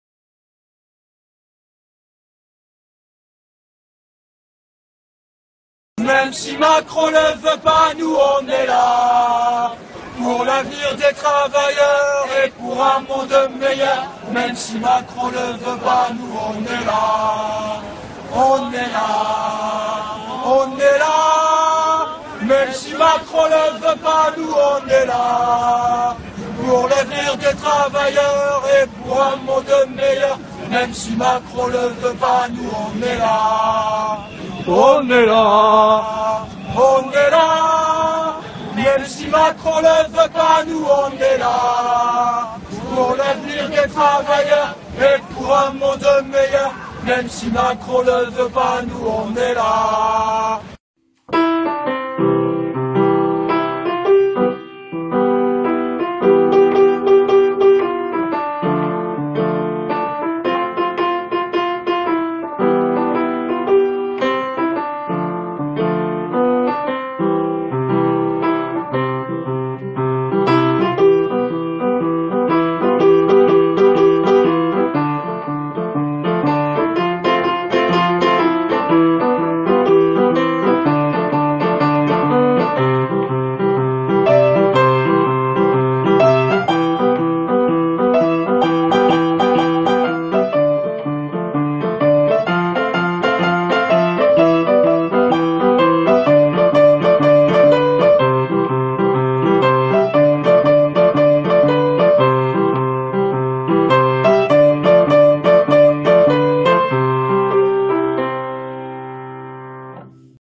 Gilets jaunes - Auxerre 89 Yonne - Piratage des décorations de Noël Diaporama 18-12-2019
Gilets jaunes - Auxerre 89 Yonne - Piratage des décorations de Noël
Diaporama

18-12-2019